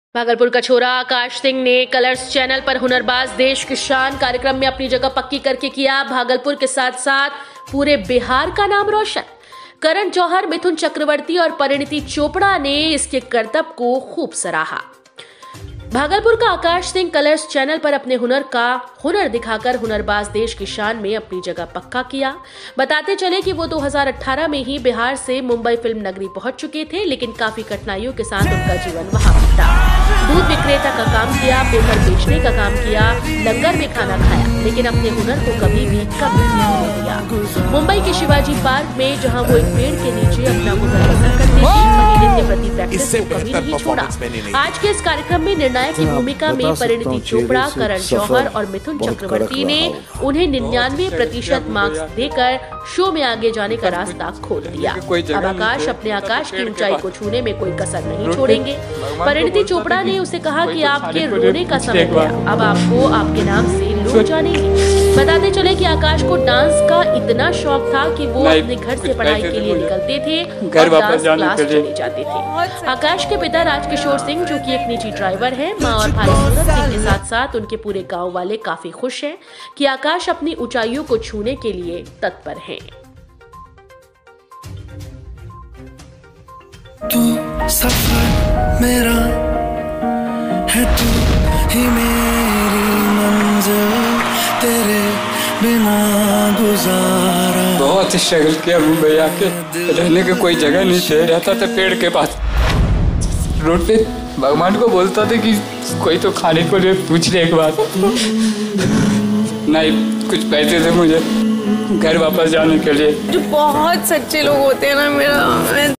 शाबाश : भागलपुर का छोरा आकाश सिंह ने भागलपुर के साथ-साथ पूरे बिहार का नाम रोशन //dancer akash singh
VIDEO : भागलपुर का छोरा आकाश सिंह ने भागलपुर के साथ-साथ पूरे बिहार का नाम रोशन*